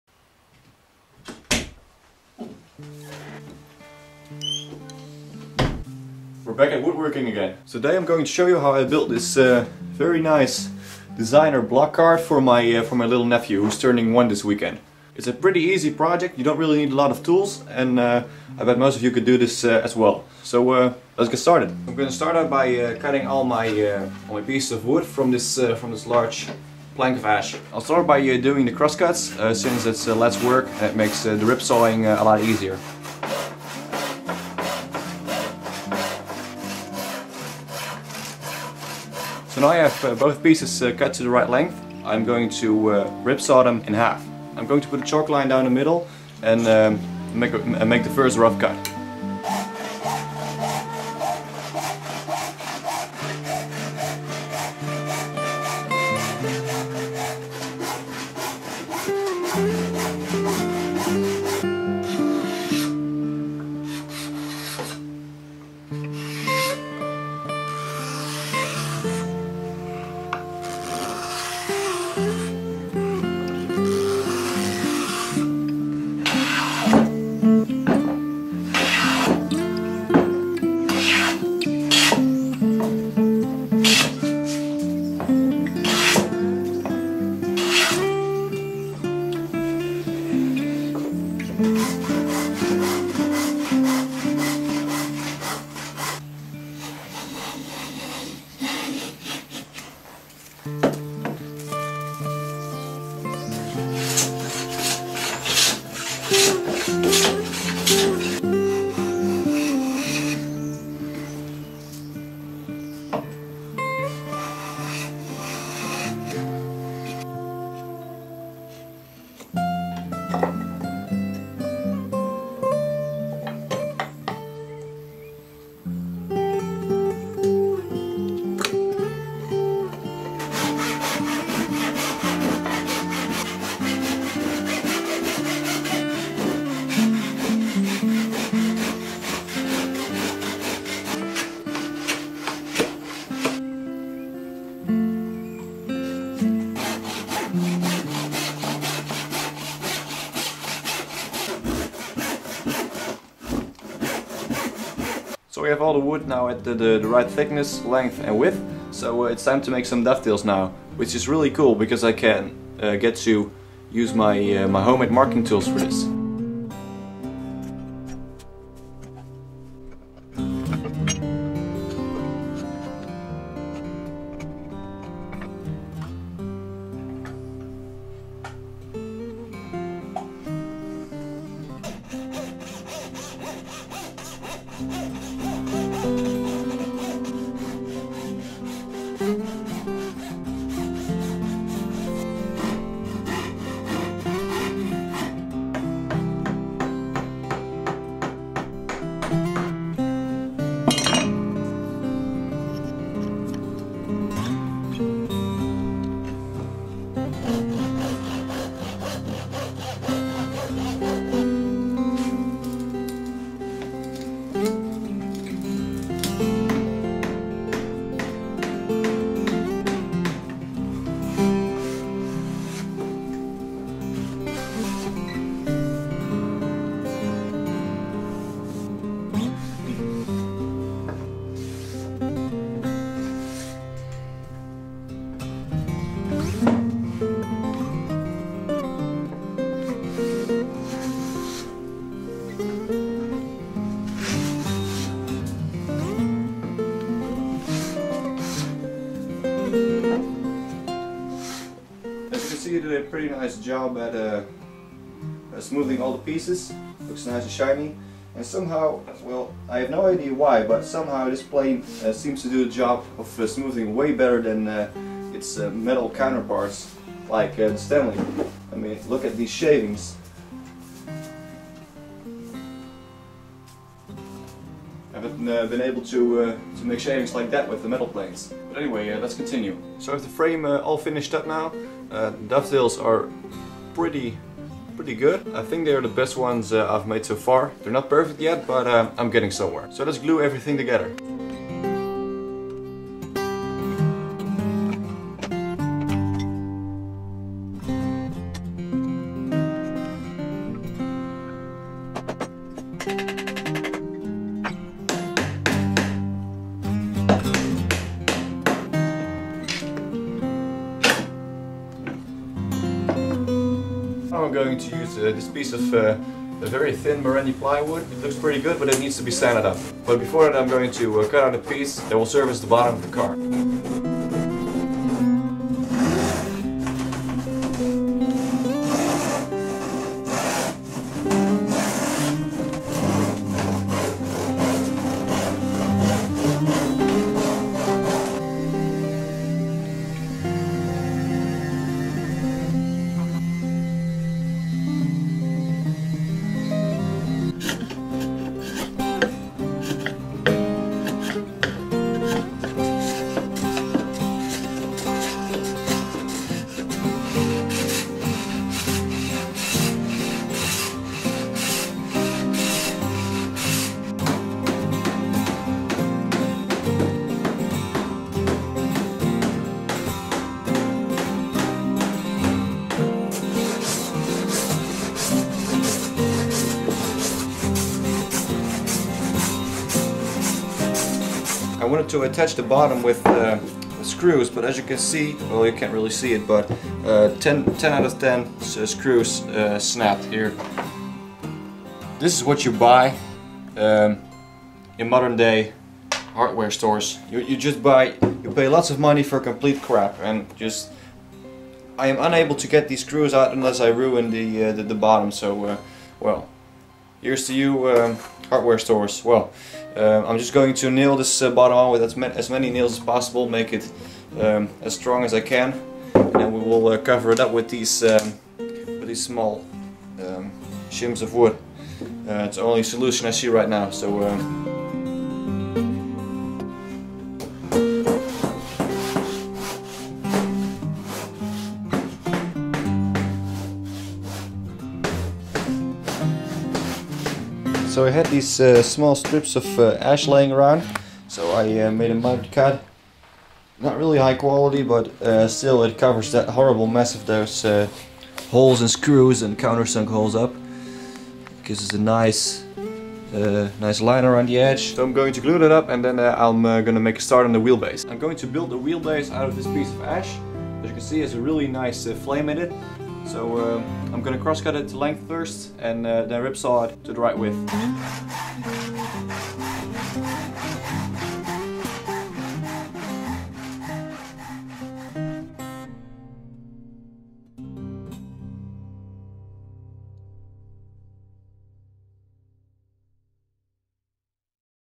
How To Make A Children's Block Cart Part 1
Today I show you how I made this block cart for my little nephew. It's a simple woodworking project most of you will be able to pull off.